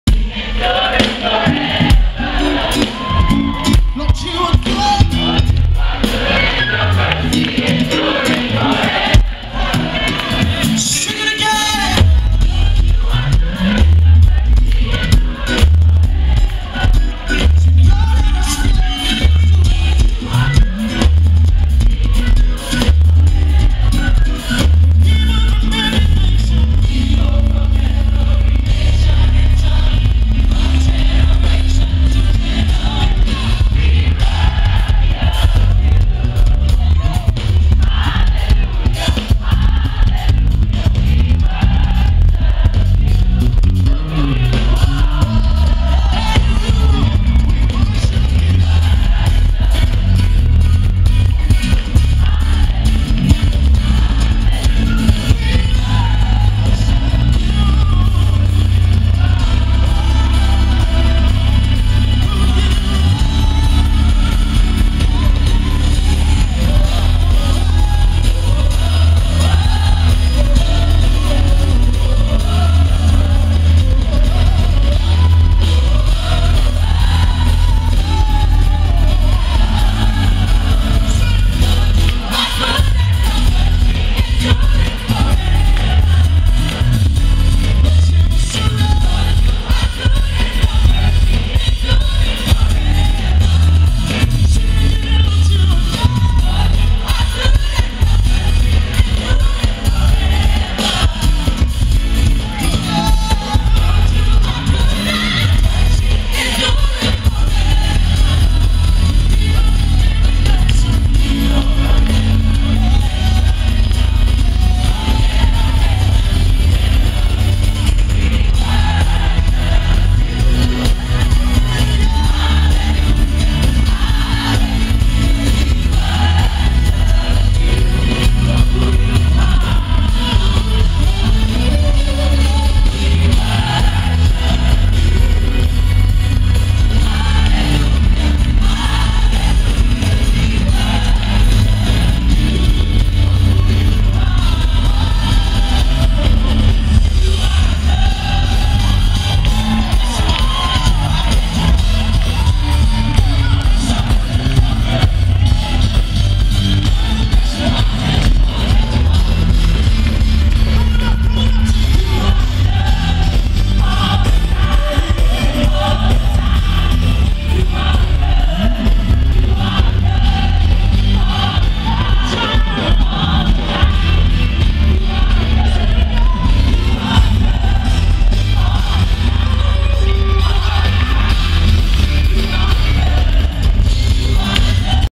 Israel Houghton
8773 - Jekalyn Carr, Israel Houghton, Tosha Cobbs at the First Annual Unity Concert Charlotte, NC 9-1-13